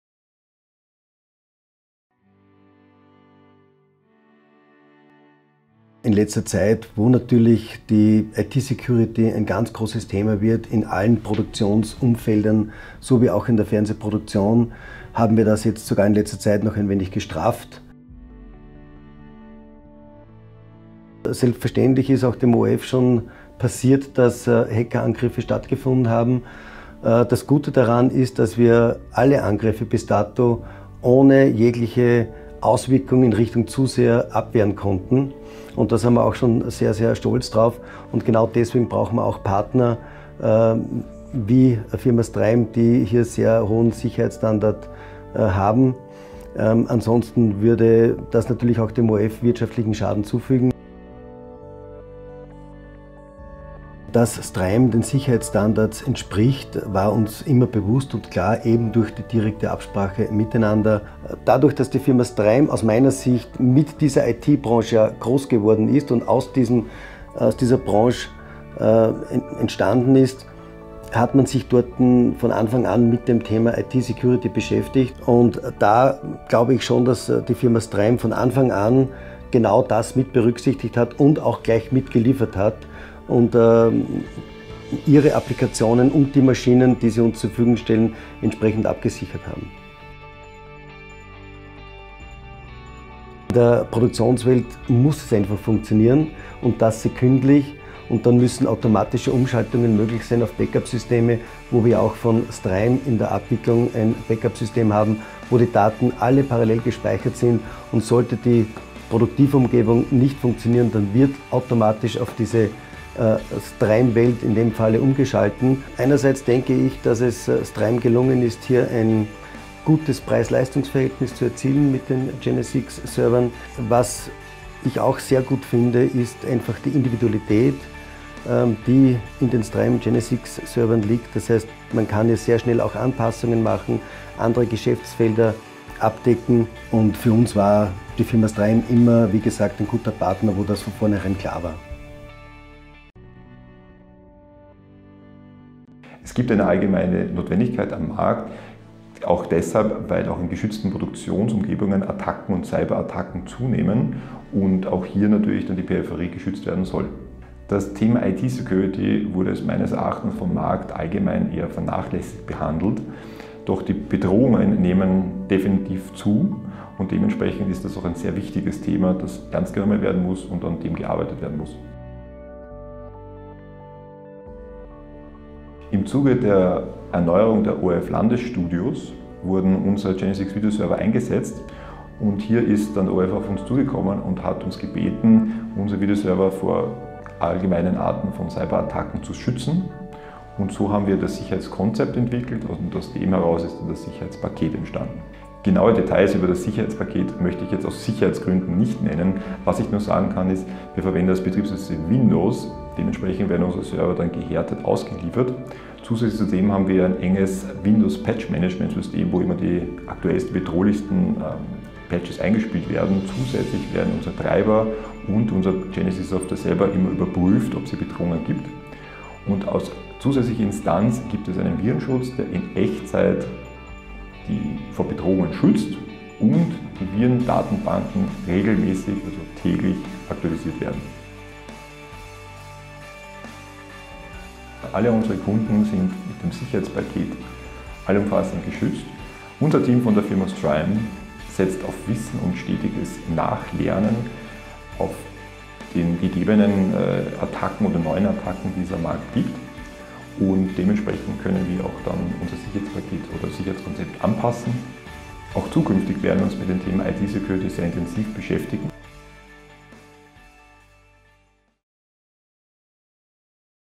IT Security for GENESIX VideoServer
Interview with Andreas Bauer (ORF, Head of Maintenance & Support) and Clemens Czepe (STRYME, Chief Technical Officer) regarding IT security in broadcast productions and Cyber Crime.

We from STRYME offer a security package including an antivirus within our GENESIX VideoServer.

It’s the first video server in the broadcast industry with built-in defences to protect from unauthorized interventions and prevent cyberattacks in real time.

We have been working as specialists for the industry, who experienced the lack of security of media houses. It is a known fact that cybercrime is on the rise. And broadcast productions are in need of safer environments.

The GENESIX Security Package is a standard feature in the video server and consists of three components (see link).